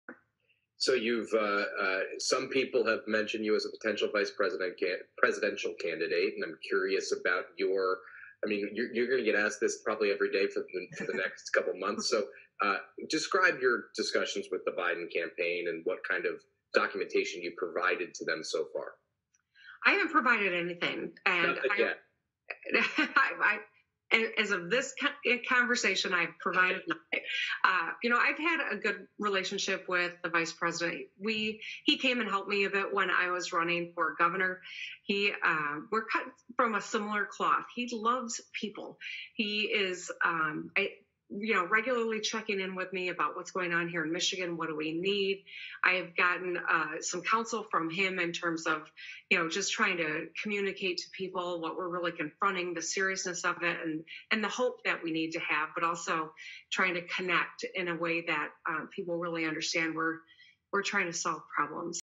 Whitmer Says Joe Biden Is Helping Her With Her Communication Strategy
Democrat Governor Gretchen Whitmer of Michigan says that 2020 Democrat presidential candidate Joe Biden is helping her with her communication strategy. From a virtual interview on Politico Playbook with hosts Anna Palmer and Jake Sherman on 4/27/2020.